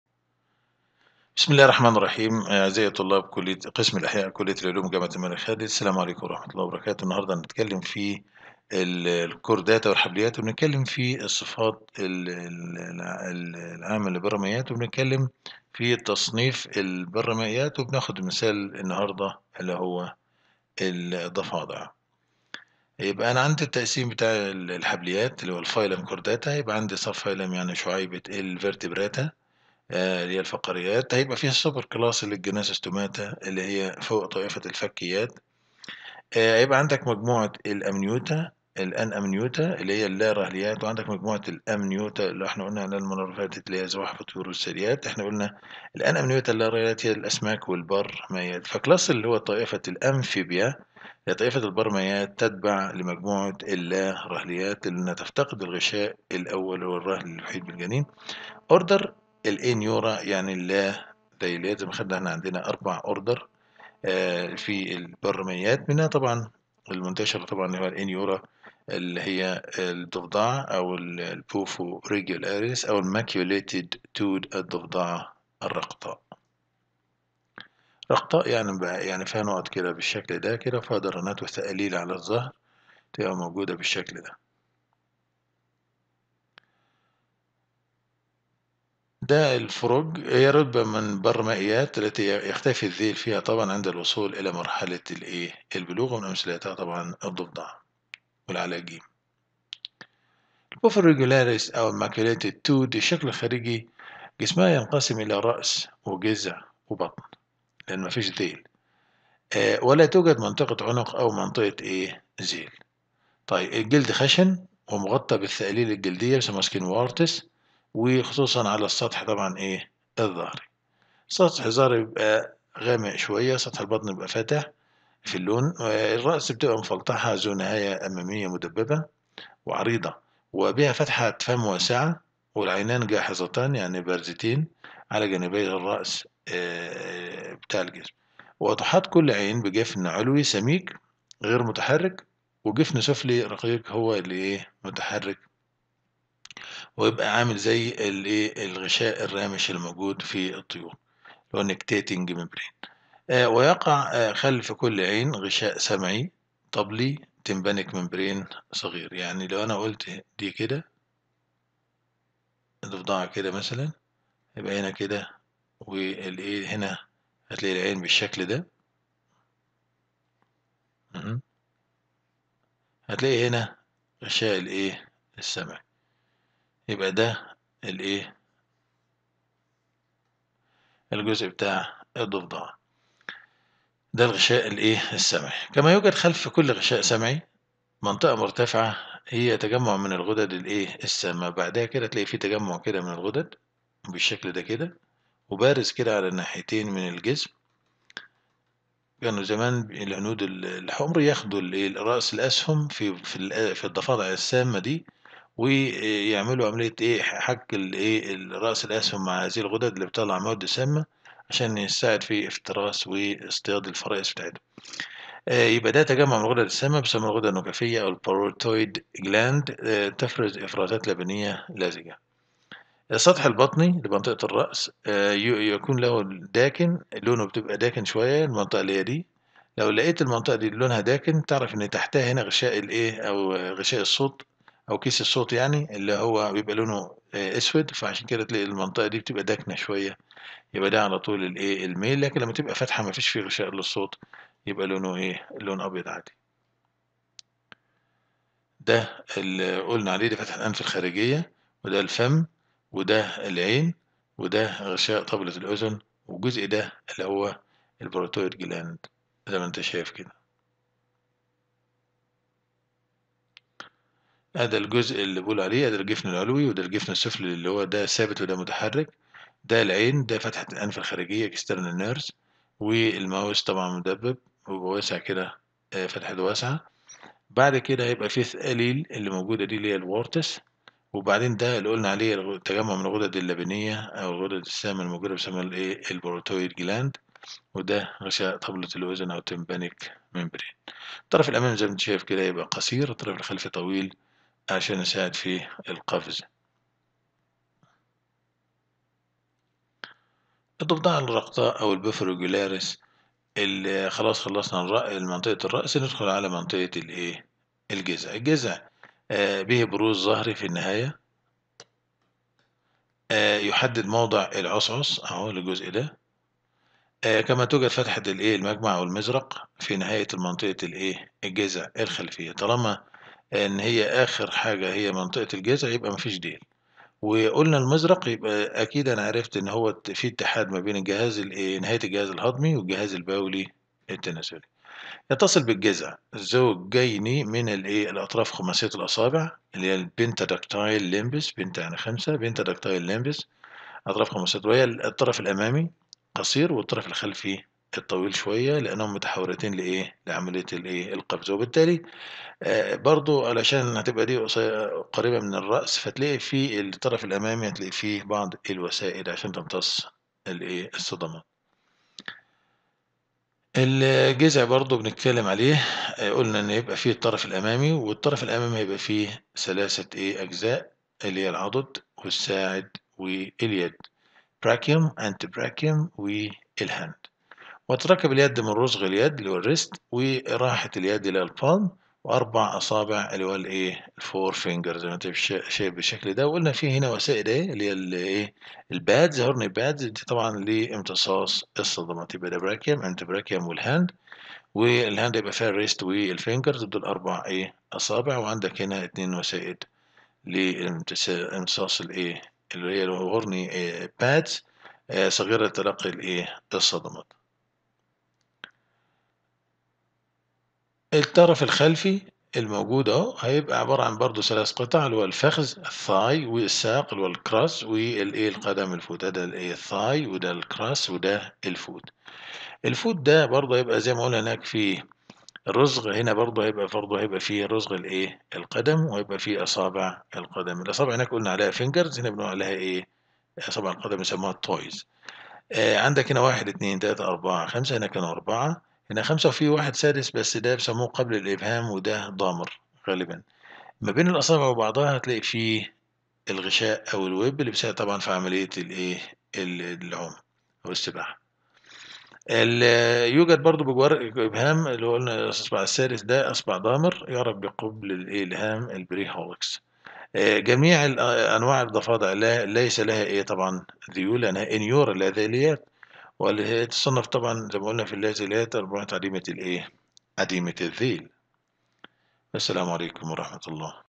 الحبليات | الوحدة 9-4 | البرمائيات - 2
المقررات المفتوحة - الحبليات - 252 حين
الوحدة 9 : رباعيات الأقدام - اللارهليات - الرهليات
الدرس 4 : البرمائيات - 2
الضفدعة الرقطاء - الشكل الخارجي